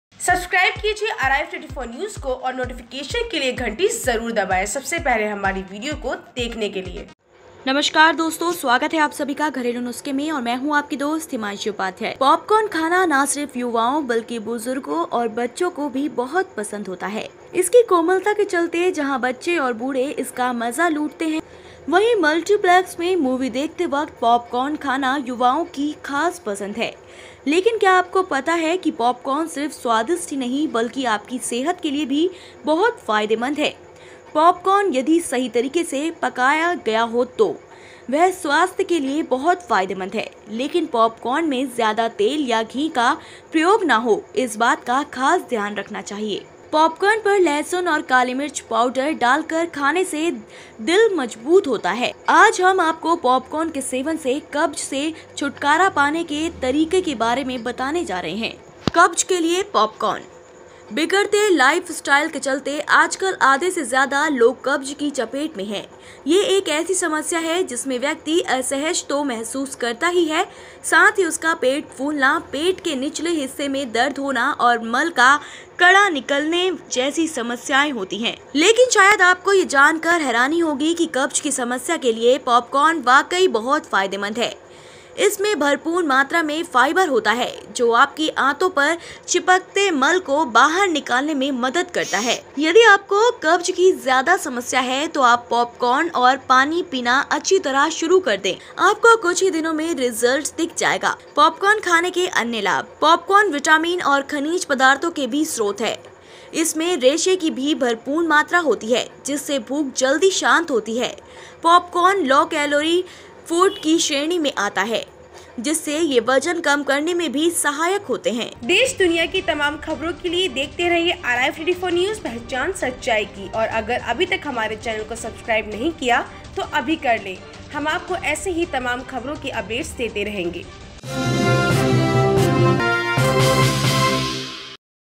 फाइबर से भरपूर है पॉपकॉर्न, इस तरह खाने से जड़ से खत्म होगी कब्ज || Arrive 24 NEWS
पॉपकॉर्न खाना न सिर्फ युवाओं बल्कि बुजुर्गों और बच्चों को भी बहुत पसंद होता है। इसकी कोमलता के चलता जहां बच्चे और बूढ़े इसका मजा लूटते हैं वहीं, मल्‍टील्‍पेक्‍स में मूवी देखते वक्‍त पॉपकॉर्न खाना युवाओं की खास पसंद है। लेकिन क्‍या आपको पता है पॉपकॉर्न सिर्फ स्वादिष्ट ही नहीं बल्कि आपकी सेहत के लिए भी बहुत फायदेमंद है। पॉपकॉर्न यदि सही तरीके से पकाया गया हो तो वह स्‍वास्‍थ्‍य के लिए बहुत फायदेमंद है। लेकिन पॉपर्कार्न में ज्‍यादा तेल या घी का प्रयोग न हो इस बात का खास ध्यान रखना चाहिए। पॉपकॉर्न पर लहसुन और कालीमिर्च पाउडर डालकर खाने से दिल मजबूत होता है। आज हम आपको पॉपकॉर्न के सेवन से कब्ज से छुटकारा पाने की तरकीब बता रहे हैं।